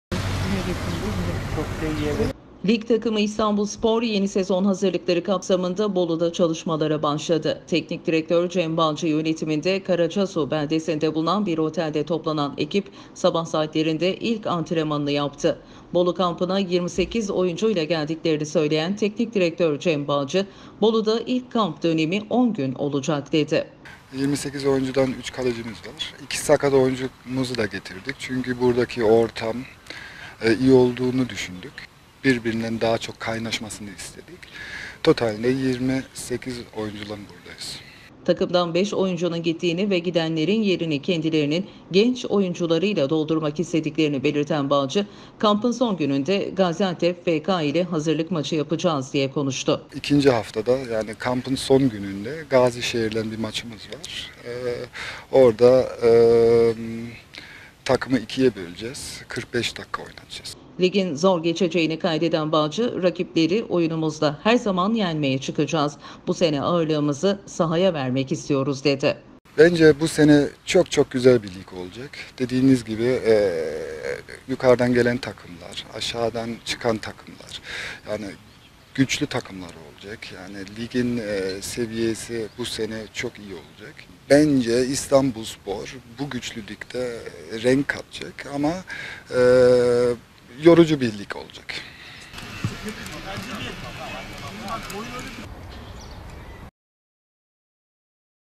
İSTANBULSPOR’UN KAMPI BAŞLADI
İstanbulspor, yeni sezon hazırlıklarına Bolu'da düzenlediği kamp ile başladı. Sezon öncesi iddialı olduklarını dile getiren Teknik Direktör Cem Bağcı, "Rakipleri oyunumuzla her zaman yenmeye çıkacağız. Bu sene sahaya ağırlığımızı vermek istiyoruz" dedi.

Lig takımı İstabulspor yeni sezon hazırlıkları kapsamında Bolu'da çalışmalara başladı. Teknik direktör Cem Bağcı yönetiminde Karacasu beldesinde bulunan bir otelde toplanan ekip sabah saatlerinde ilk antrenmanını yaptı. Bolu kampına 28 oyuncuyla geldiklerini söyleyen Teknik Direktör Cem Bağcı, "Bolu'da ilk kamp dönemi 10 gün olacak" dedi.
Takımdan 5 oyuncunun gittiğini ve gidenlerin yerini kendilerinin genç oyuncularıyla doldurmak istediklerini belirten Bağcı, "Kampın son gününde Gaziantep FK ile hazırlık maçı yapacağız" diye konuştu.
Ligin zor geçeceğini kaydeden Bağcı, "Rakipleri oyunumuzla her zaman yenmeye çıkacağız. Bu sene ağırlığımızı sahaya vermek istiyoruz" dedi.